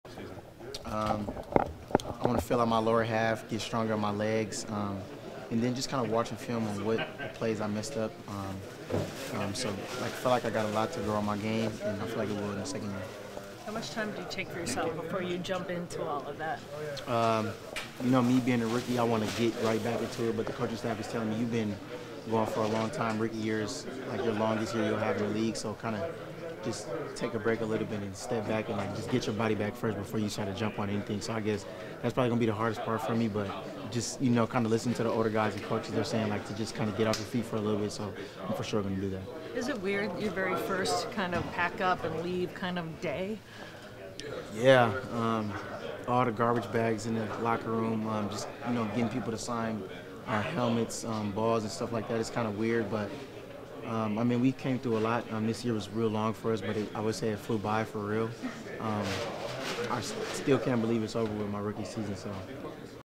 Mekhi Blackmon on Goals For His First NFL Offseason & Dealing With 2023 Season Ending
Minnesota Vikings cornerback Mekhi Blackmon addressed the media from the TCO Performance Center.